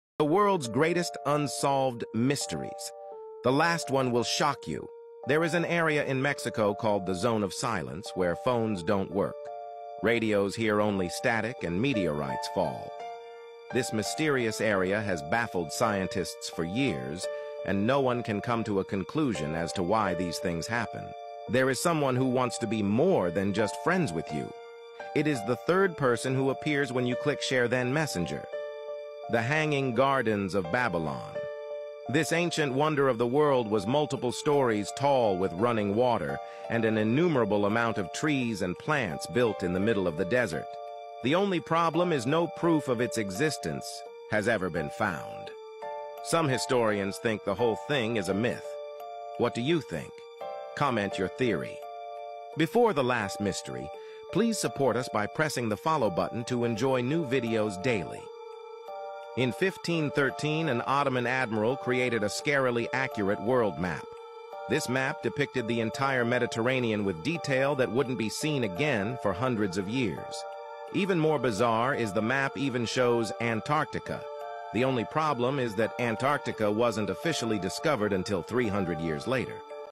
The World's Greatest Unsolved Mysteries! 🔍
Dive into the enigmatic world of the greatest unsolved mysteries that have baffled experts and curious minds alike! From ancient artifacts to unexplained phenomena, this video explores the most intriguing cases that remain a mystery.

               EVERYTHING

Join us as we delve into these fascinating mysteries, revealing theories, expert opinions, and compelling evidence that could change everything we think we know. Whether you're a fan of true crime, history, or the paranormal, there's something here for everyone!

Watch now and embark on a journey through the unknown!